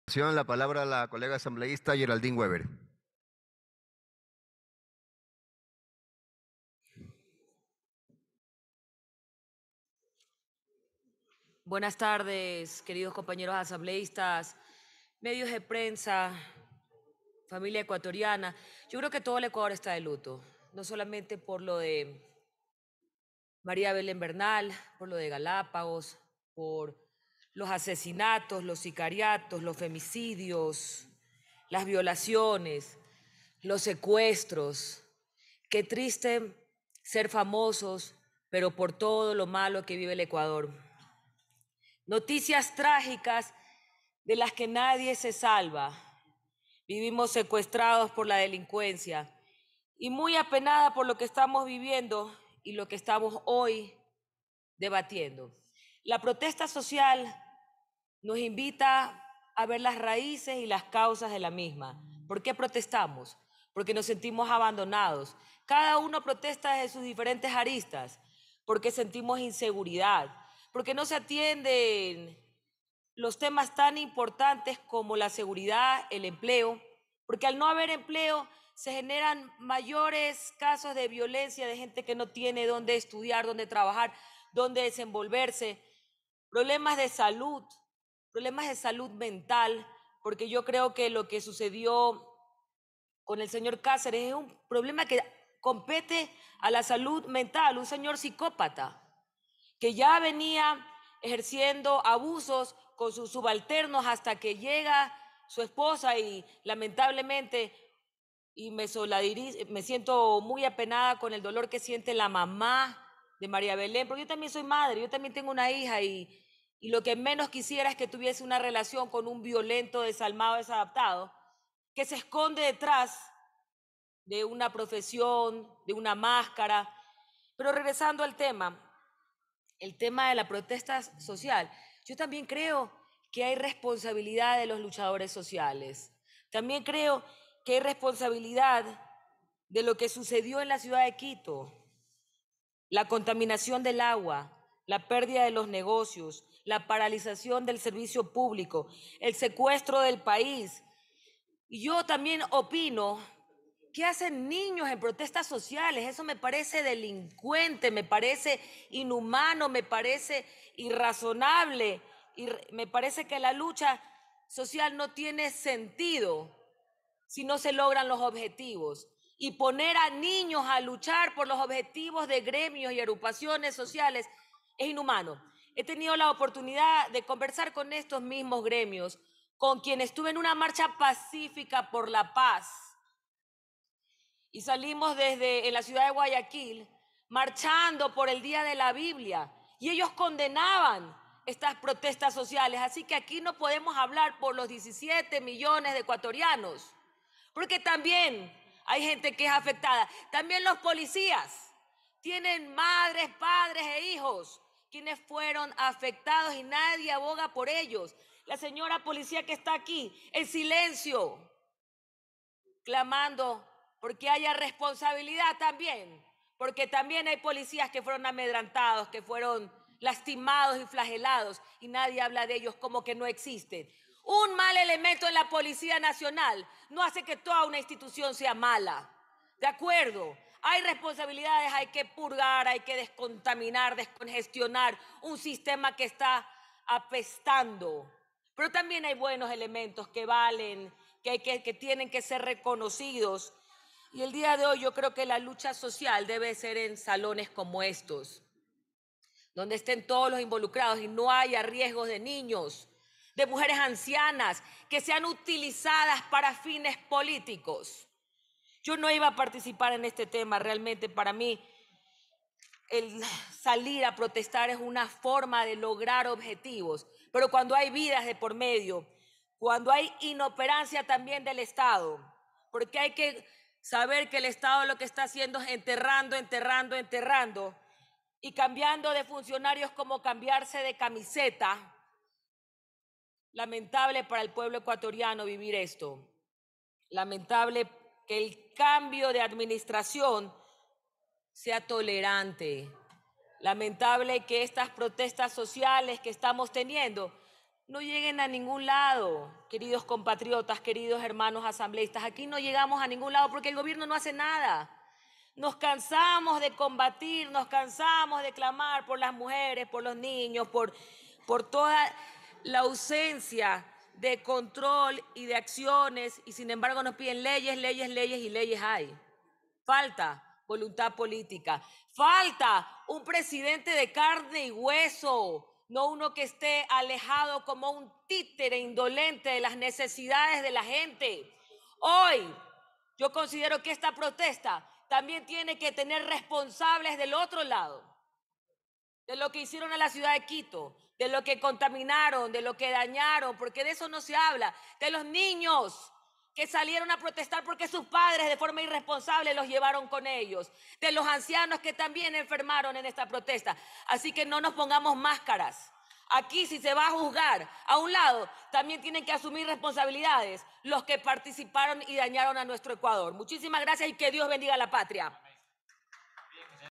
Asambleísta Geraldine Weber - Sesión 799 - ParoNacionalEc2022
27 de septiembre de 2022
Informe del Proceso de Fiscalización y Control Político, respecto a los hechos relacionados con el Paro Nacional del 13 al 30 de junio de 2022, sobre las medidas adoptadas durante las jornadas de protesta, los hechos de violencia que constituyen infracciones y los acuerdos establecidos entre el gobierno y las organizaciones indígenas y sociales.